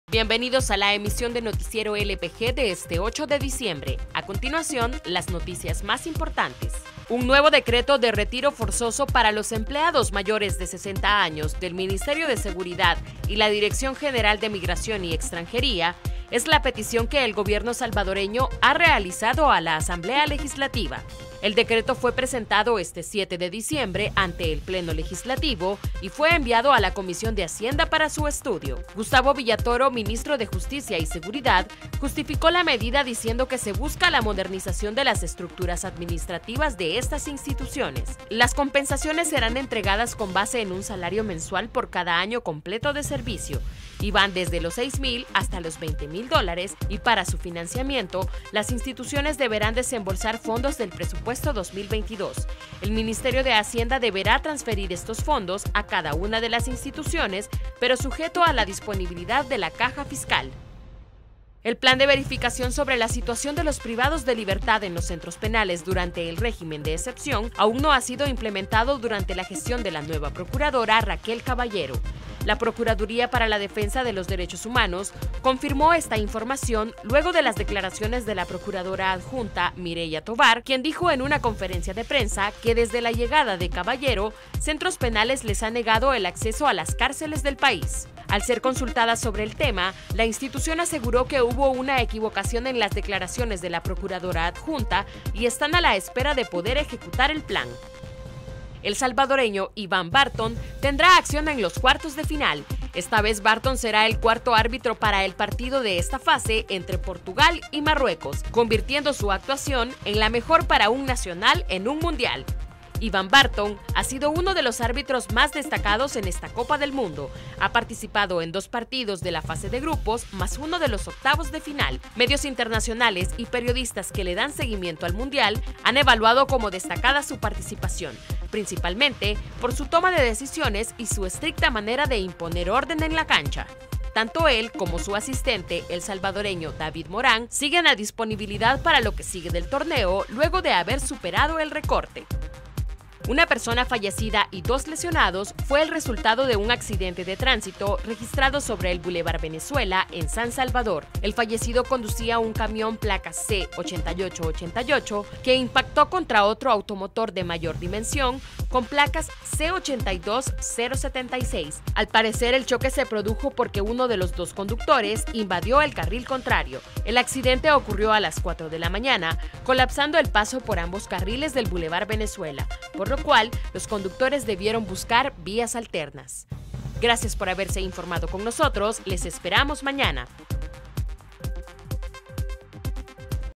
Noticiero LPG 08 de diciembre: Gobierno pide nuevo retiro forzoso para Ministerio de Seguridad
Gustavo Villatoro, ministro de Justicia y Seguridad, justificó la medida diciendo que se busca la "modernización de las estructuras administrativas" de estas instituciones.

¡Visítenos!